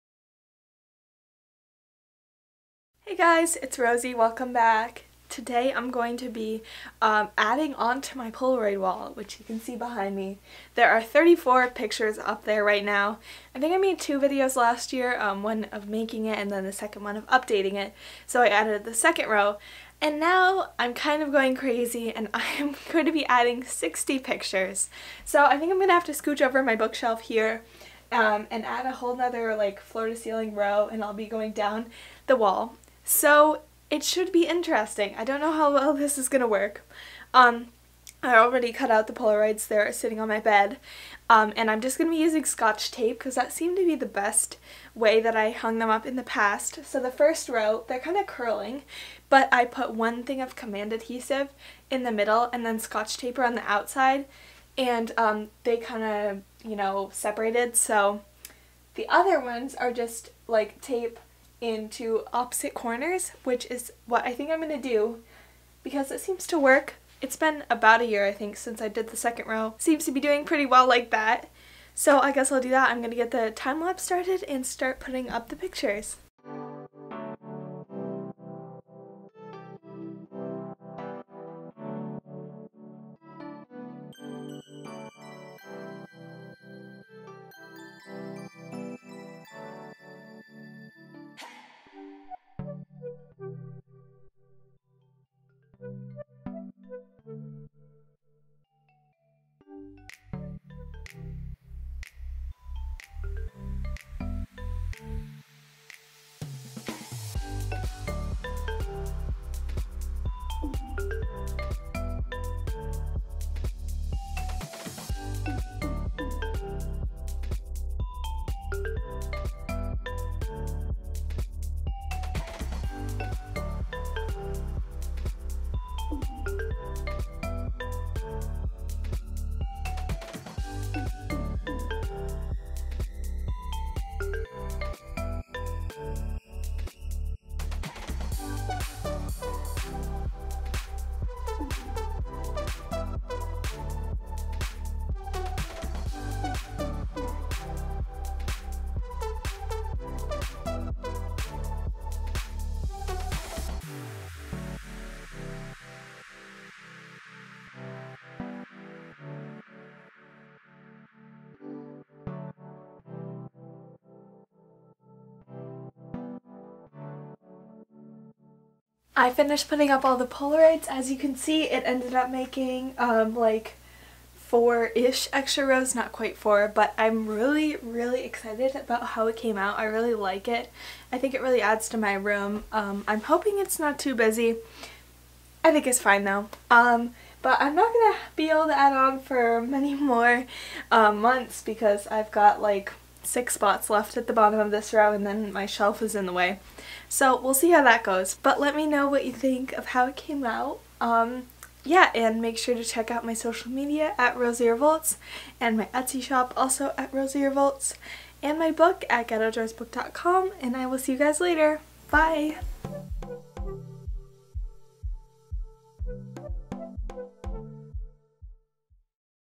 Updating my Polaroid Wall 2020
Watch me update my Polaroid wall again with photos from 2020!